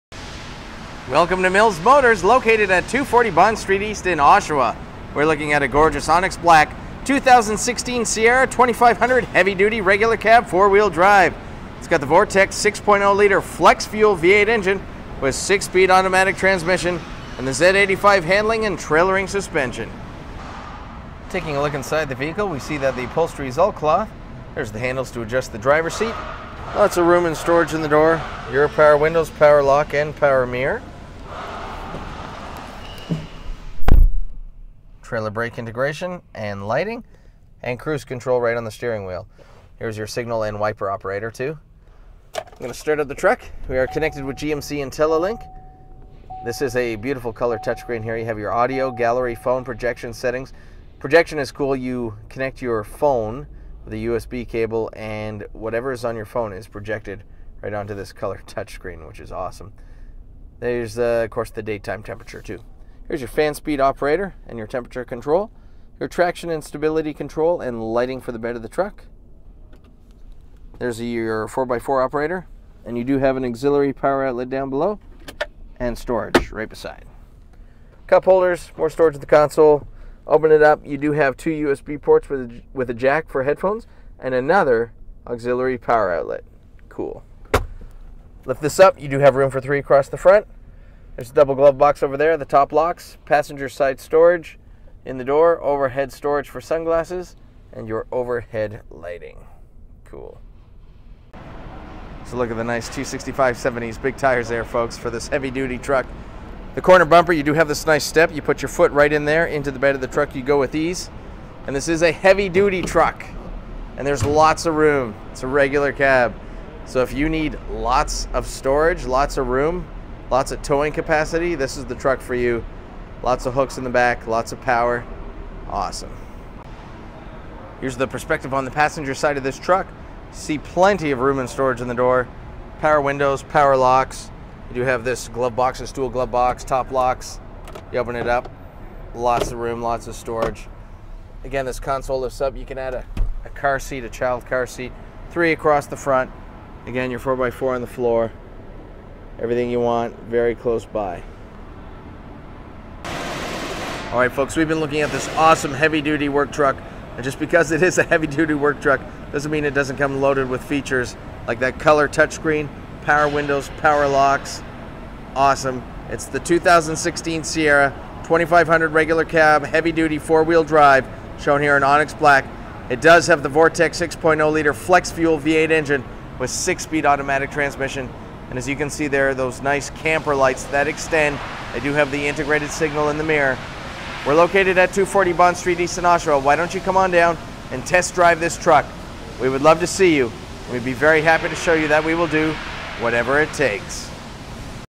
2016 GMC Sierra 2500HD Regular Cab 4WD Z85 Handling Trailering Suspension Oshawa ON Stock # 160461
This is a 2016 GMC Sierra 2500HD with 6-Speed A/T transmission Black[Onyx Black] color and Jet Black interior color.  This video is recorded and uploaded by cDemo Mobile Inspector